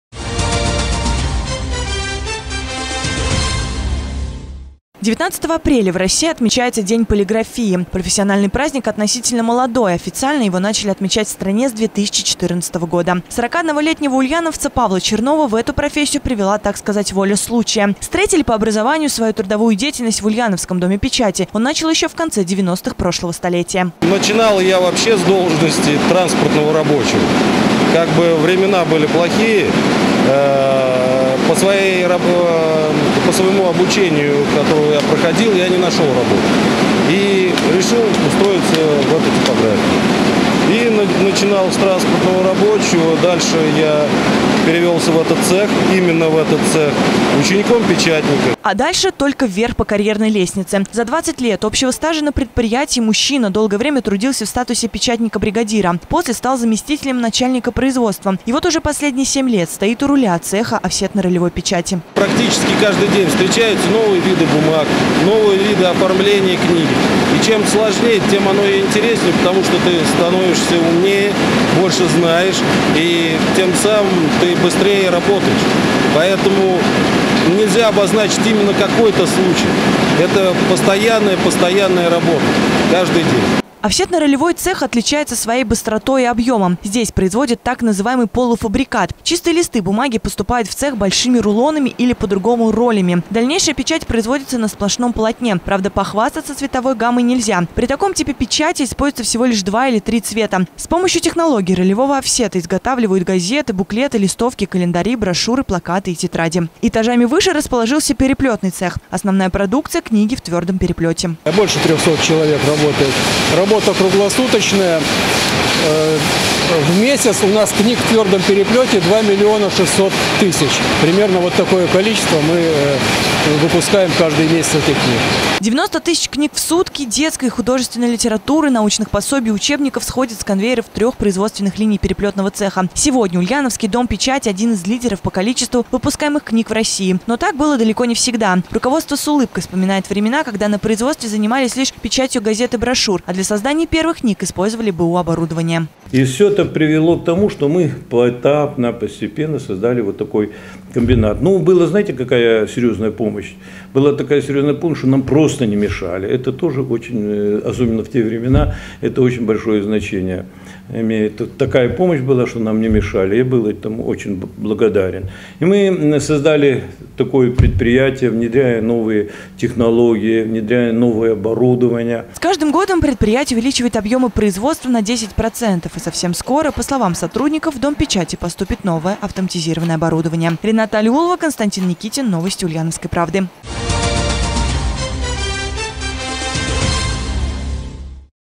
Из офсета до ролей. Ульяновские полиграфисты отмечают профессиональный праздник
19 апреля в России отмечается День полиграфии. Профессиональный праздник относительно молодой, официально его  начали отмечать в стране с 2014 года.